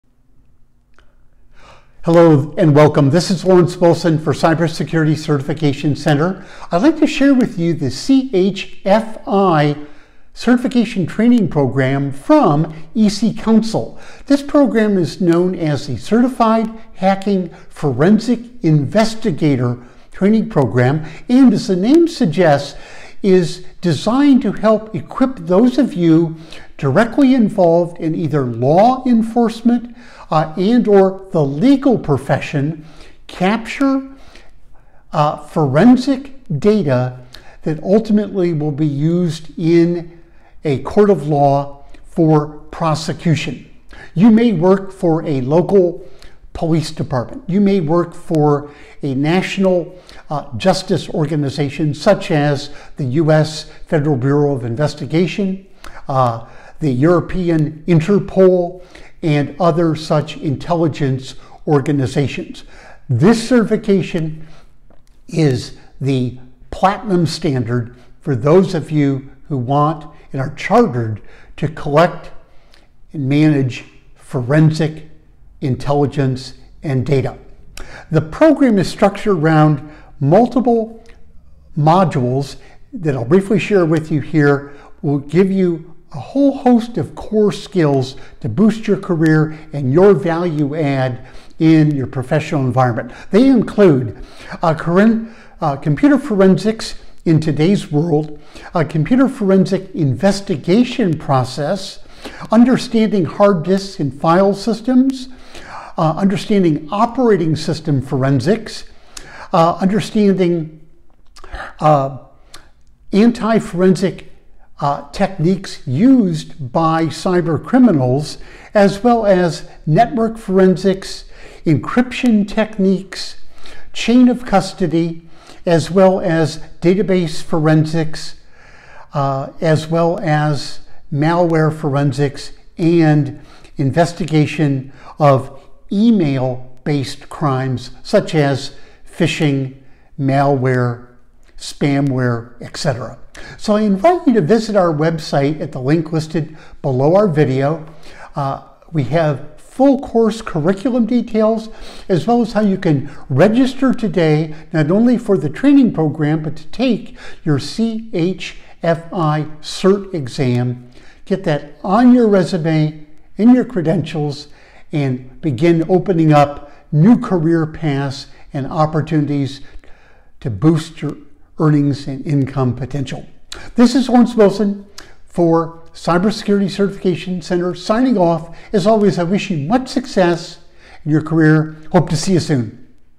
Certified Hacking Forensic Investigator Training – EC Council (CHFI [Exam 312-49]
InfoSec & Cyber colleagues, computer forensics is the application of computer investigation and analysis techniques in the interests of determining potential legal evidence. Evidence might be sought in a wide range of computer crime or misuse, including but not limited to theft of trade secrets, theft of or destruction of intellectual property, and fraud. CHFI investigators can draw on an array of methods for discovering data that resides in a computer system, or recovering deleted, encrypted, or damaged file information known as computer data recovery. The purpose of the CHFI credential is to validate the candidate’s skills to identify an intruder’s footprints and to properly gather the necessary evidence to prosecute in the court of law. Training modules cover: 1) Computer Forensics in Today’s World, 2) Computer Forensics Investigation Process, 3) Understanding Hard Disks and File Systems, 4) Operating System Forensics, 5) Defeating Anti-Forensics Techniques, 6) Data Acquisition and Duplication, 6) Network Forensics, 7) Investigating Web Attacks, 8) Database Forensics, 9) Cloud Forensics, 10) Malware Forensics, 11) Investigating Email Crimes, 11) Mobile Forensics, and finally 12) Investigative Reports. The 312-49 Exam has 150 multiple choice questions over 4 hours.

Much career success, Lawrence Wilson – Cybersecurity Certification Center